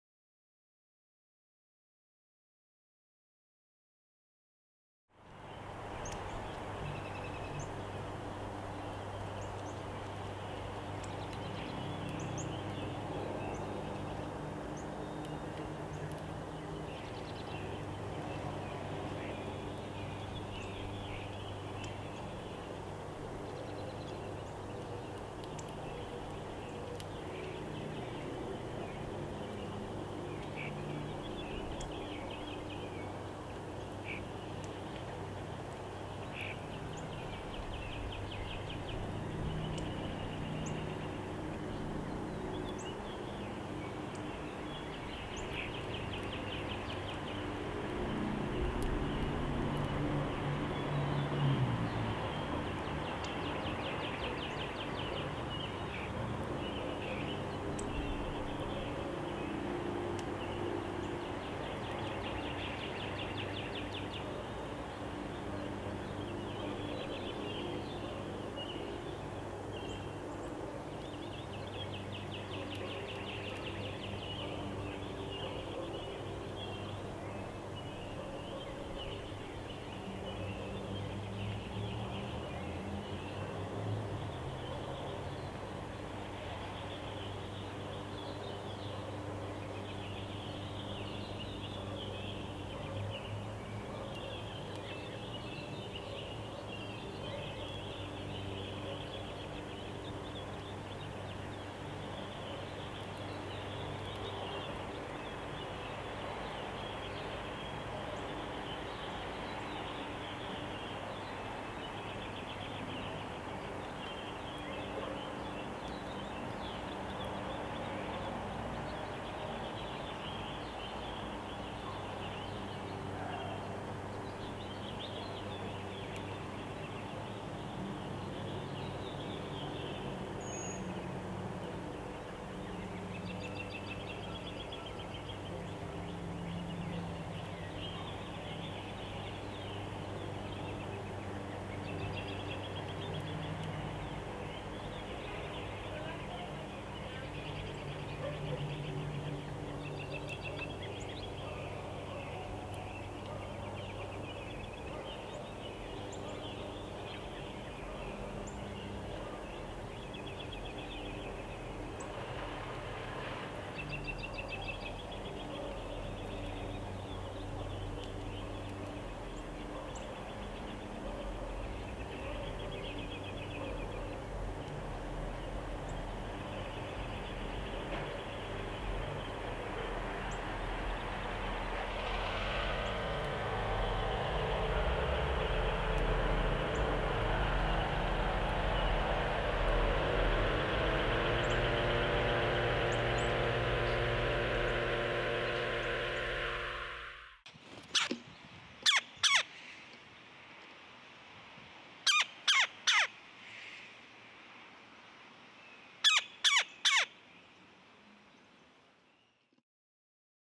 Northern Cardinal eating at gazebo feeder
A male Northern Cardinal eating at my gazebo-style feeder in the evening. The feeder contained black-oil and striped sunflower seeds, safflower seeds and peanut pieces.